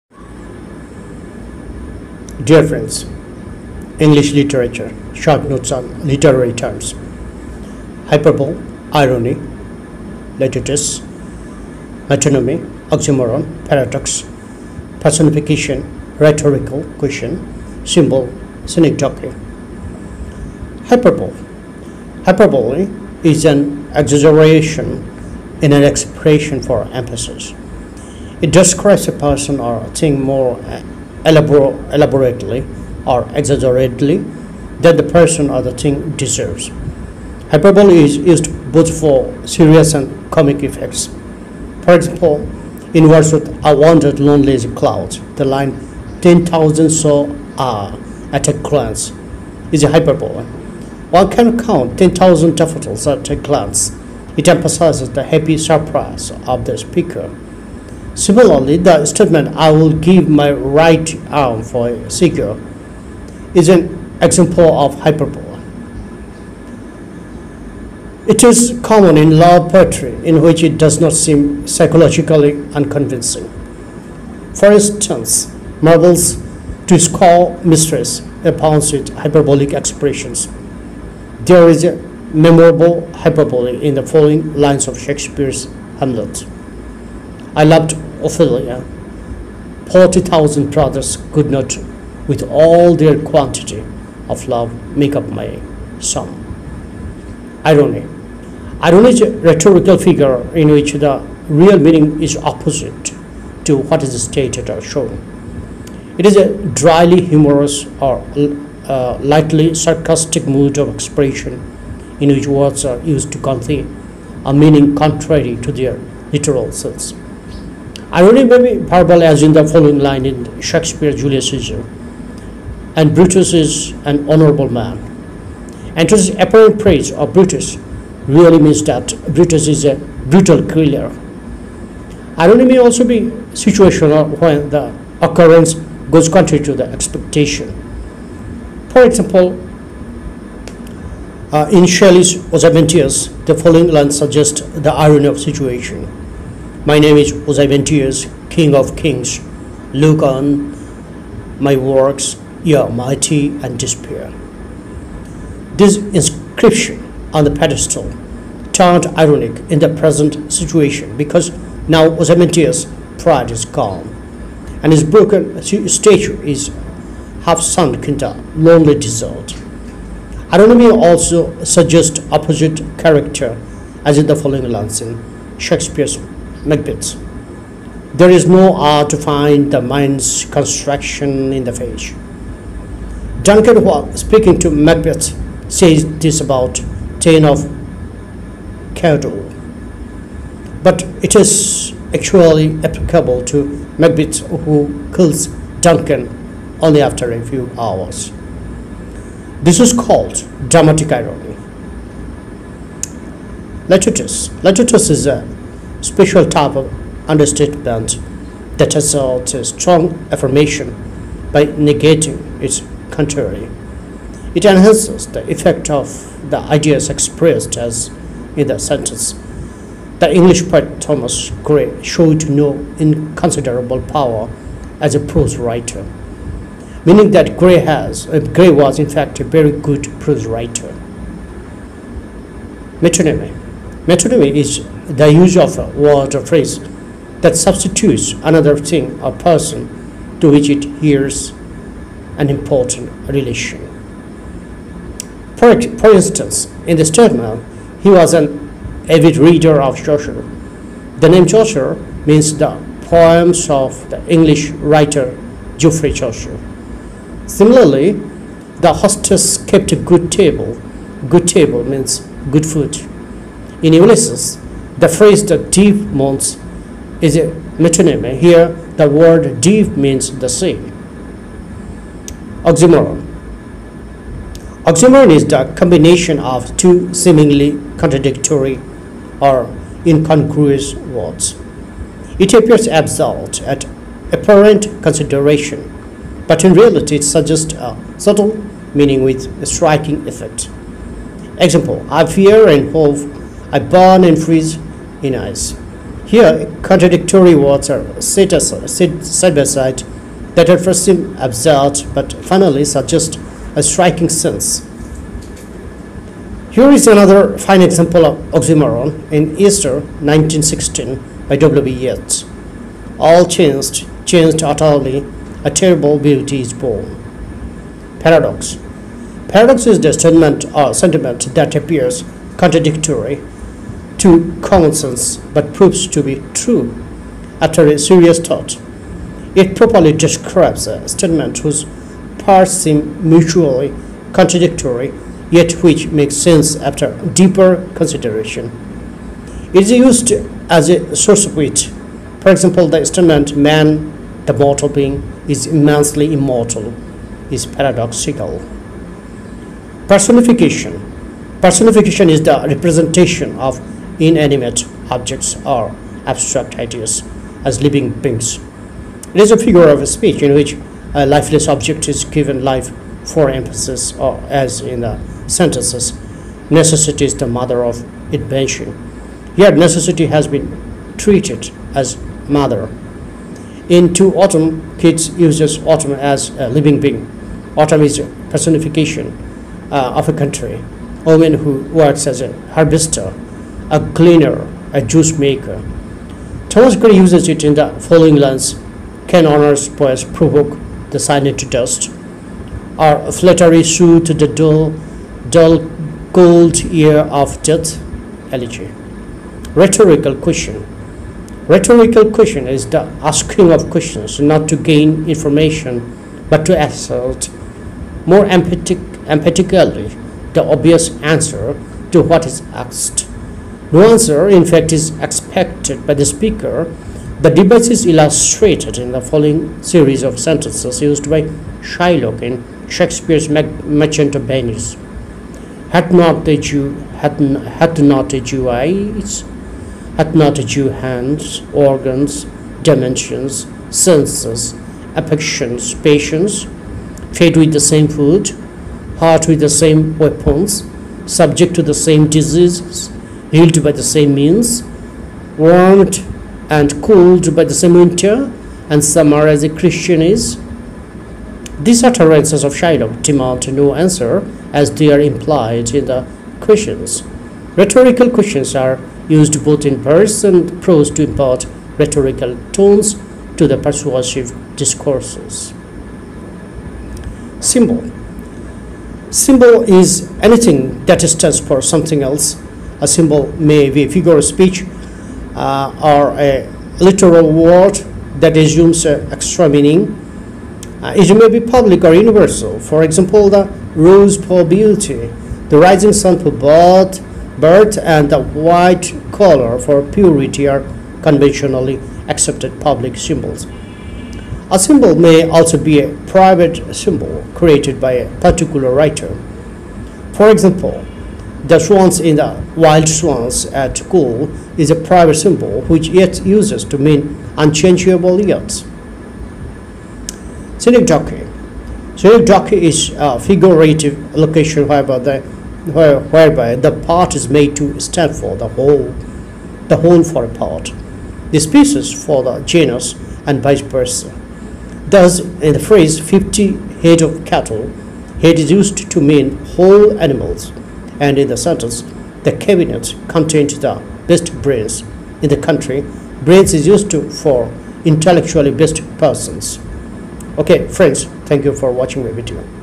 Hyperbole Irony Litotes Metonymy Oxymoron Paradox Personification Rhetorical Symbol Synecdoche
English Literature Short Notes on Literary Terms - Hyperbole,  Irony,  Litotes, Metonymy, Oxymoron, Paradox, Personification, Rhetorical question, Symbol, Synecdoche